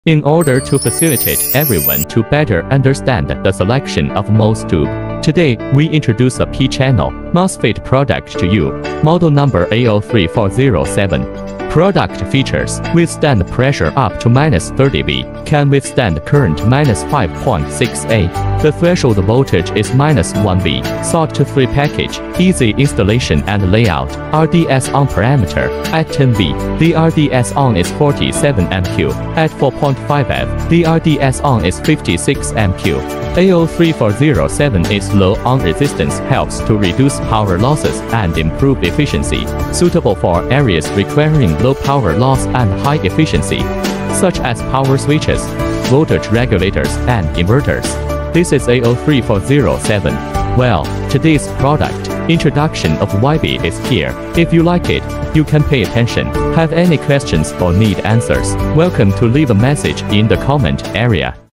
AO3407 Alternative MOSFET Parameter
AO3407 (VB2355) Parameters: Type: P-channel Drain-Source Voltage: -30V Drain Current: -5.6A On-resistance: 47mΩ@10V, 56mΩ@4.5V Gate-Source Voltage Range: 20V (±V) Threshold Voltage: -1V Package: SOT23 Application Overview: AO3407 is suitable for power switching and voltage regulation applications as a P-channel MOSFET. Its low on-resistance helps reduce power losses and improve efficiency. Advantages and Applications: Low on-resistance, suitable for applications requiring low power loss and high efficiency, such as power switches, voltage regulation, and inverters.